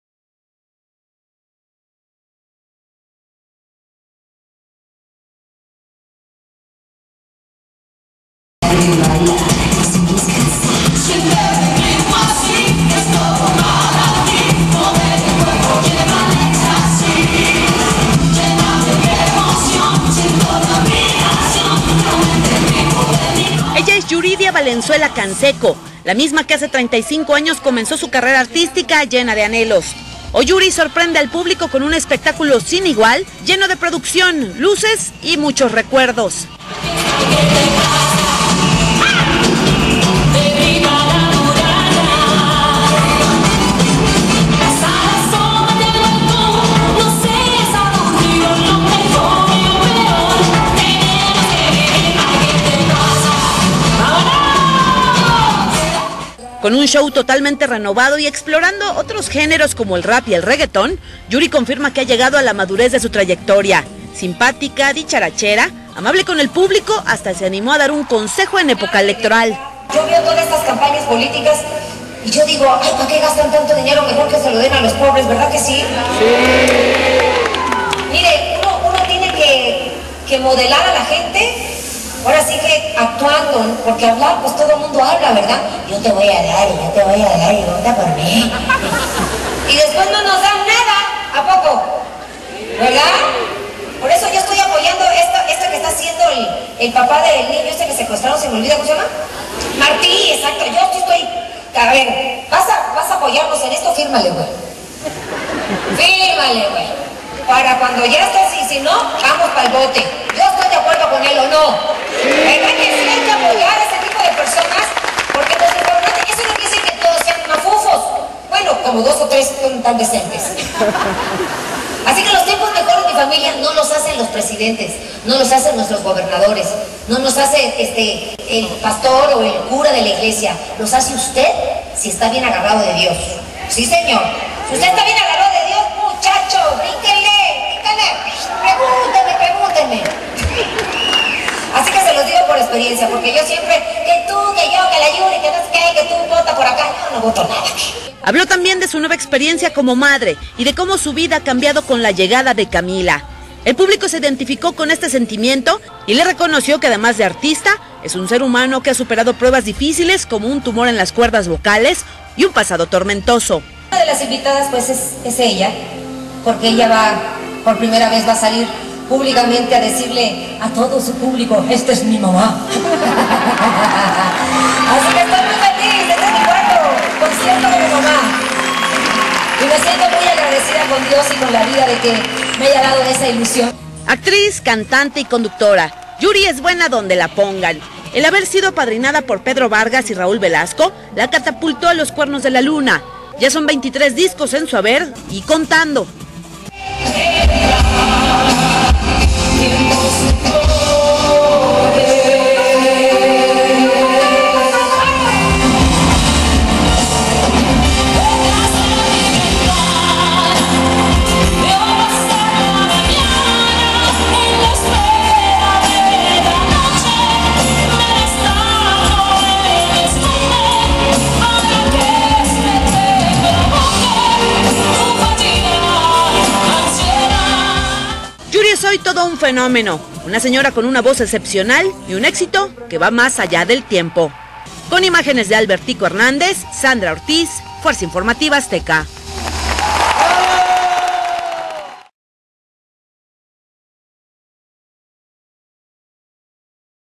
Yuri En Puebla Auditorio Siglo XXI 18-jUNIO-09 Reseña
AQUI LES DEJO UN VIDEO DEL PASADO CONCIERTO DE YURI QUE OFRECIO EN MI CIUDAD, AL CUAL ASISTI, PERO PROHIBIERON LA ENTRADA DE CAMARAS. ASI QUE EN UN CANAL LOCAL DE MI CIUDAD DIERON LA NOTA DE LA TRIUNFAL PRESENTACION DE ESTE CONCIERTO.
COSA QUE NO ENTIENDO ES POR QUE PROHIBEN TOMAR FOTOS Y VIDEOS DE UNA VIDEOCAMARA, Y DE CELULARES SI ?